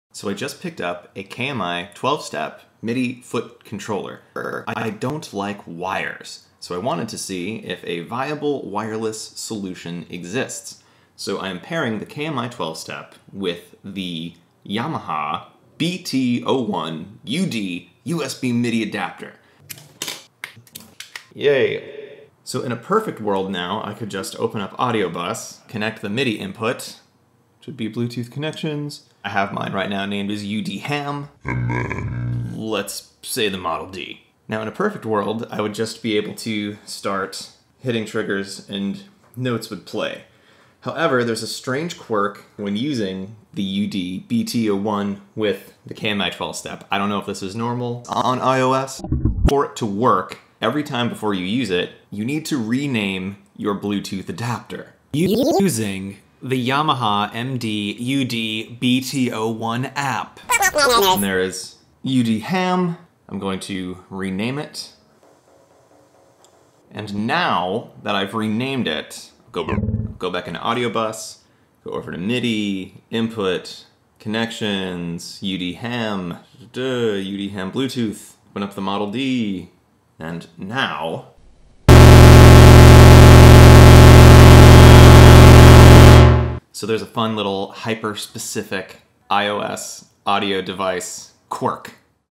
KMI 12-Step MIDI Foot Controller with Yamaha UD-BT01 Bluetooth Adapter iOS Setup Quirk
Discovered a weird trick to using KMI footpedals with bluetooth adapters on iOS. Haven't come across much posting on the subject, so I thought I'd share!